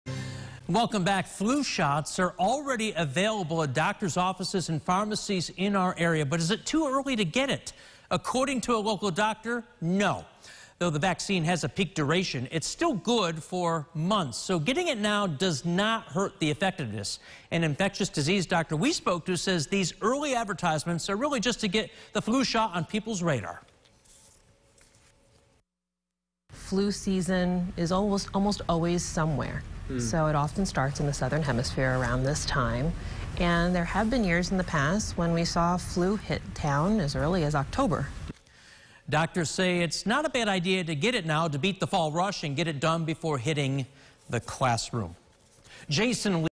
Is it too early to get the flu shot?
Flu shots are already available at doctor's offices and pharmacies in our area, but is it too early to get?